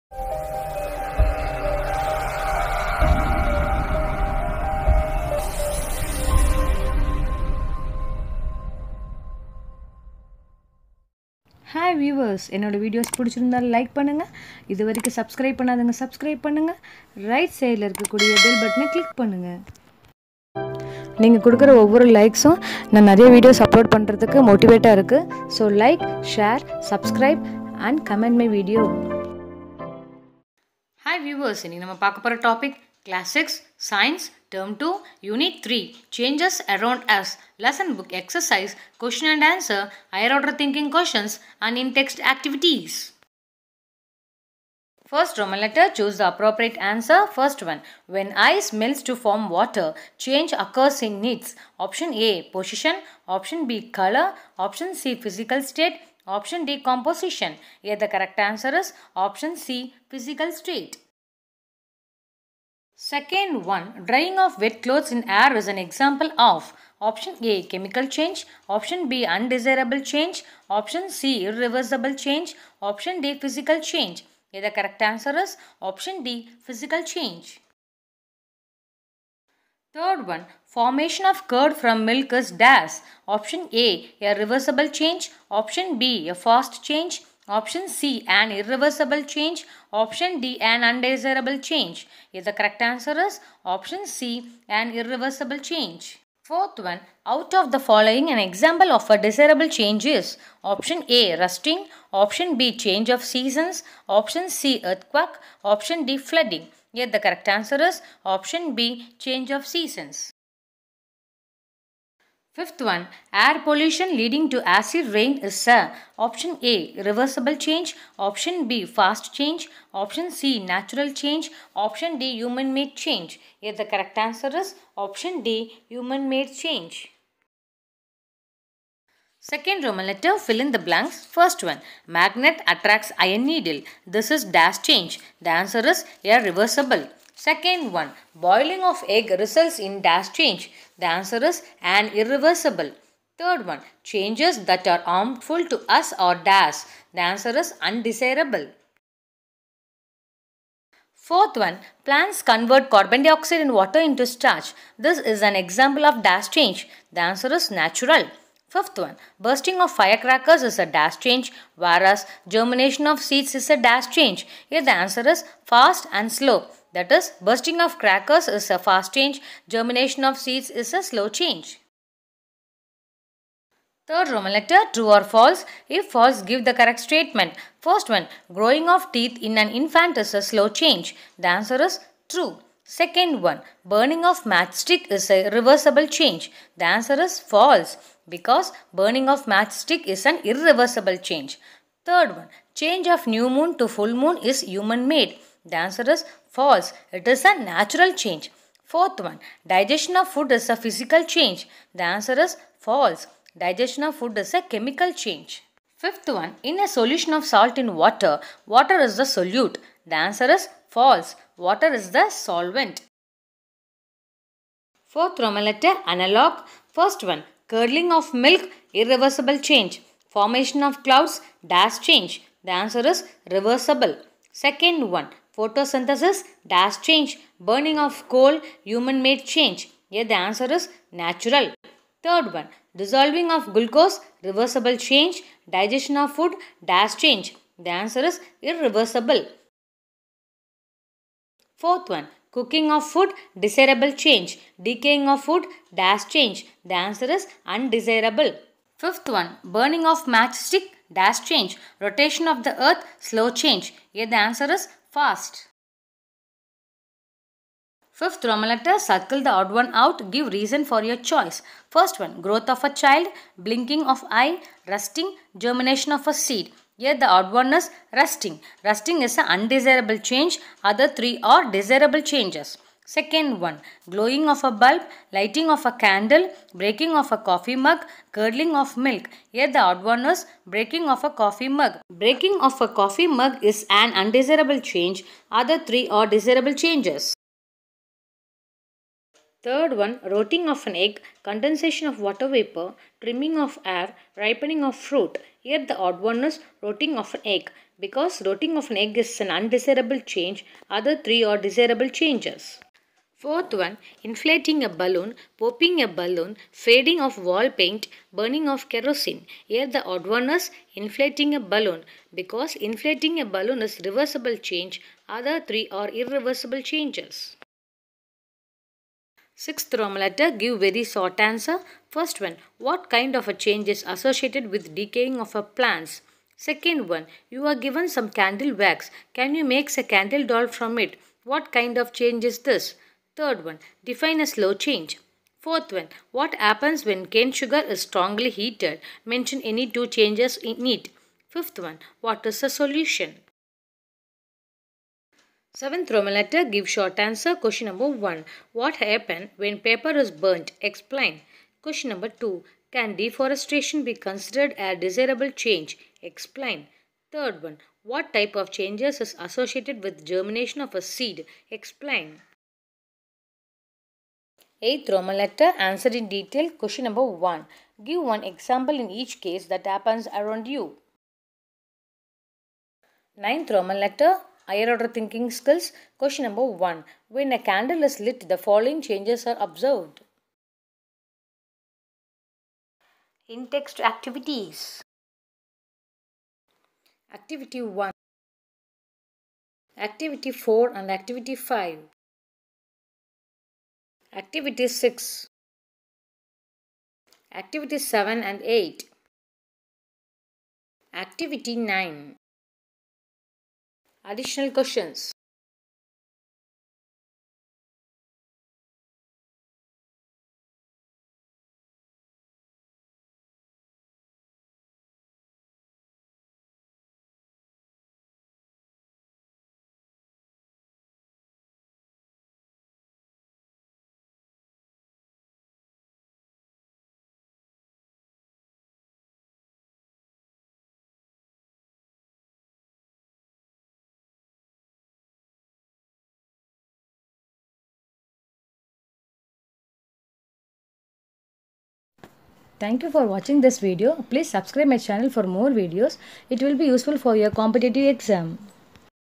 CHANGES AROUND US CLASS 6 SCIENCE TERM 2 UNIT 3 BOOK EXERCISE,QA,HOTS & INTEXT ACTIVITIES-EXTRA
Book Exercise
* True or false if false give the correct statement
* Analogy
* Circle the odd one ..Give reason for your choice
* Give very short answer
* Give short answer
* Answer in detail
* Higher order thinking questions

* Intext activity